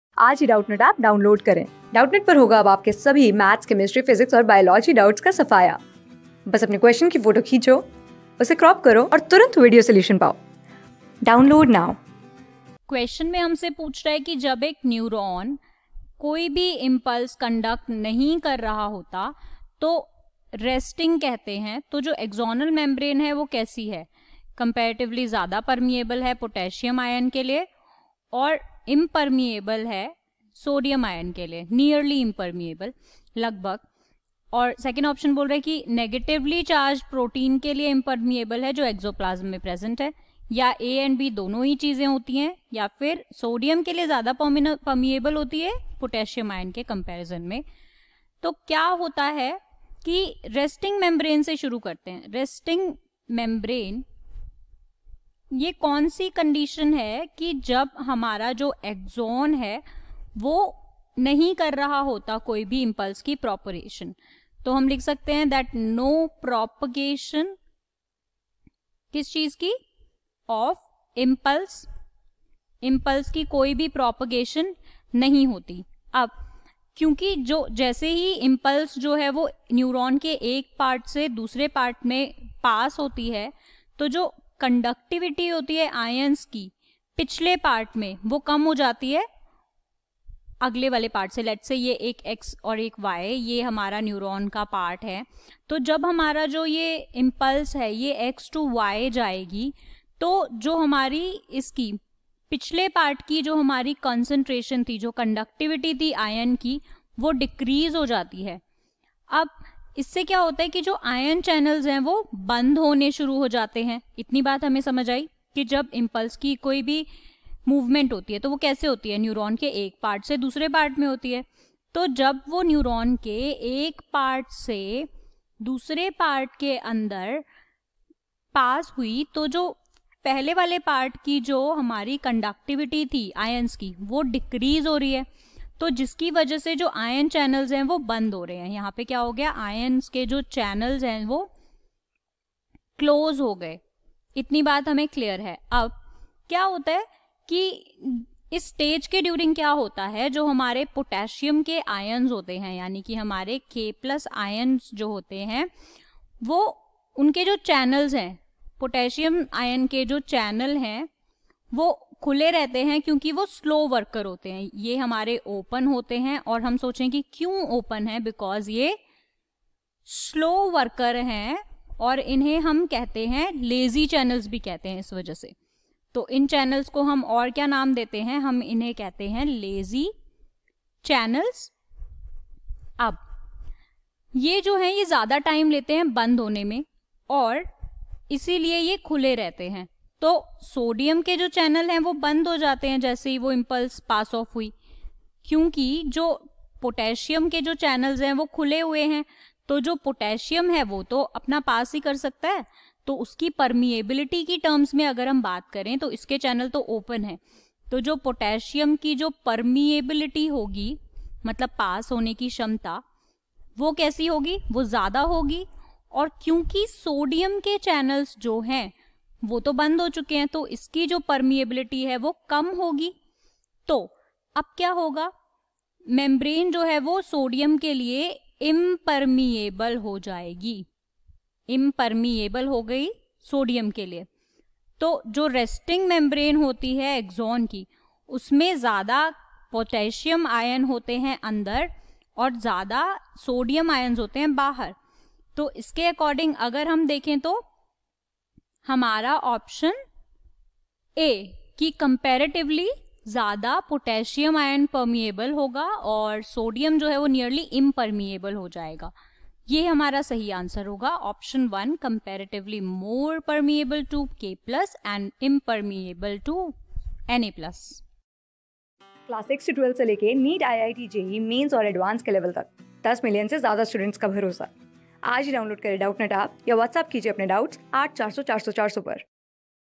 When a neuron is not conducting any impulse i.e. resting , the axonal membrance is | CLASS 12
When a neuron is not conducting any impulse i.e. resting , the axonal membrance is
Class: 12
Subject: BIOLOGY
Chapter: NEUTRAL AND COORDINATION
Board:IIT JEE

👉 Have Any Query? Ask Us.